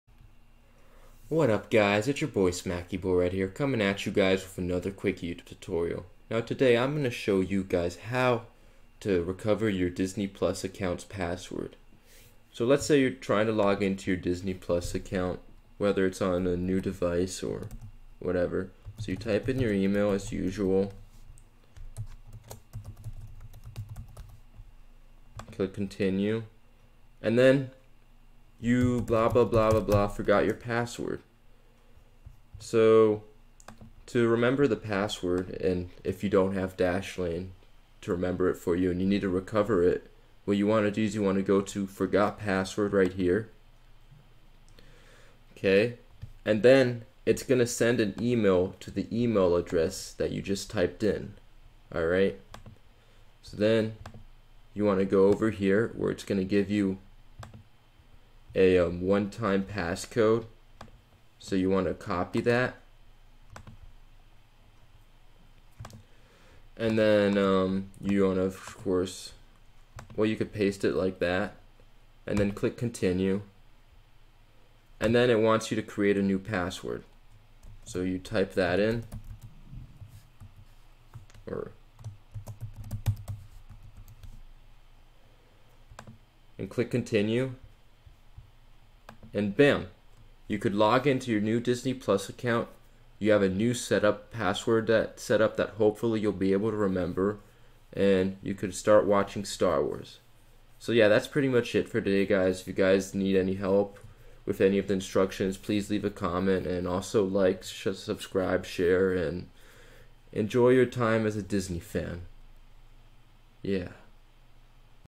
How to recover a forgotten password on Disney+
In this video, I show you guys how to recover a forgotten Disney+ password of yours.